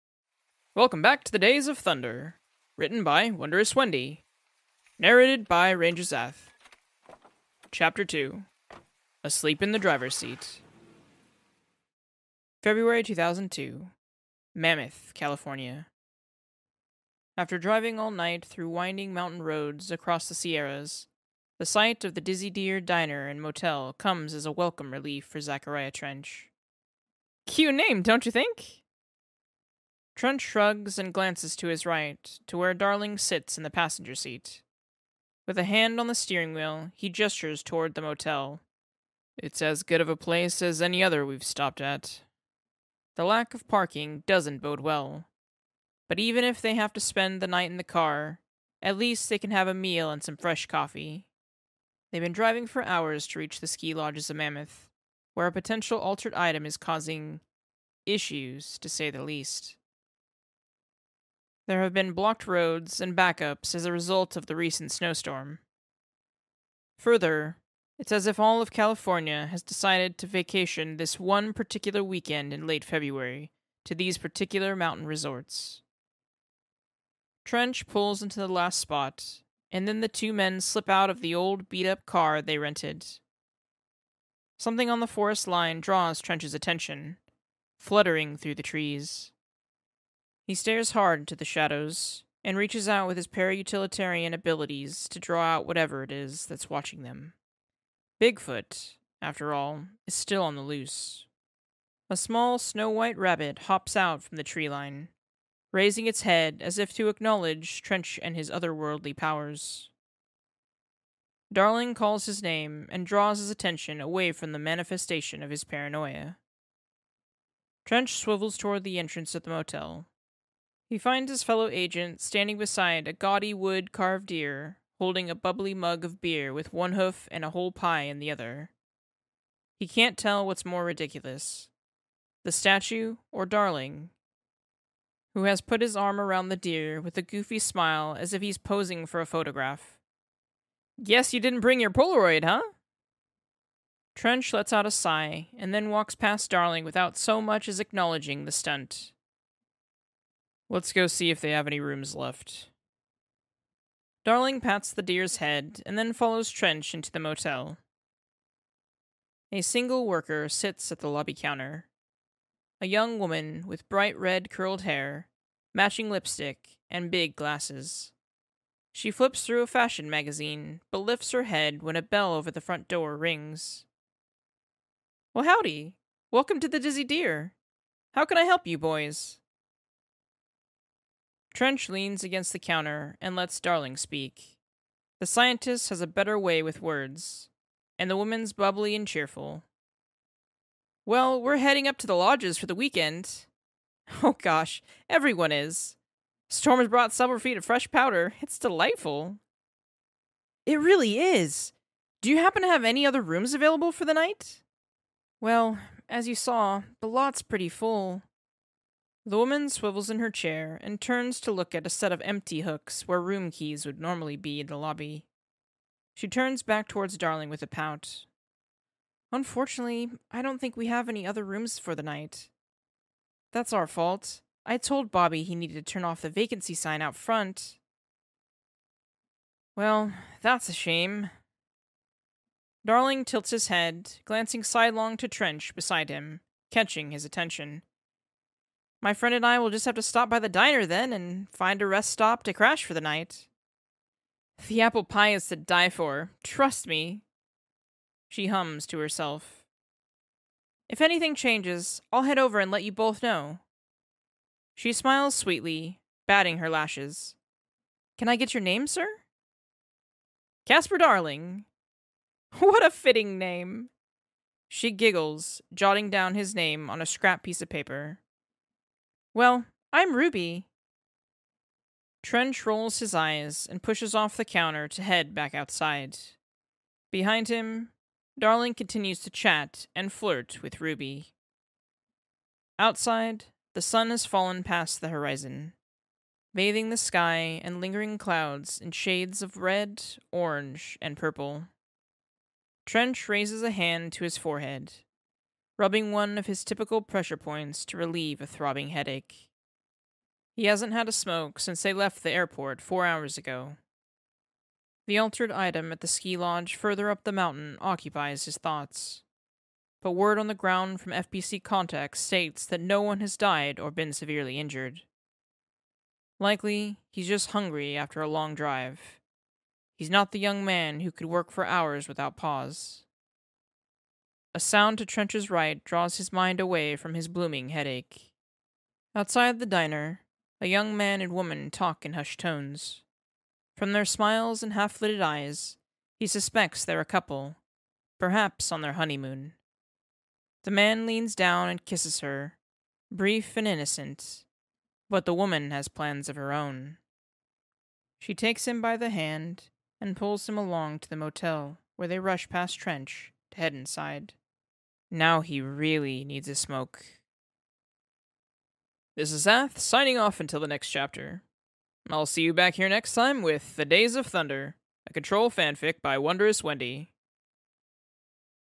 Control | The Days of Thunder | Chapter 02 [Podfic]
❗  Rating:
Explicit

♥️ Relationship:
Casper Darling/Zachariah Trench

📑 Story Summary:
AI55-PE, the Hot Tub, is known for being one of the Bureau's most infamous altered items. The item isn't known for its inherent danger to others like the Game Hammer or the Arctic Queen, but instead for its ability to induce lewd psychedelic hallucinations featuring historical figures.

To Director Zachariah Trench and Doctor Casper Darling, the winter road trip to investigate AI55-PE marked the last time both men were truly happy before the Ordinary AWE changed everything.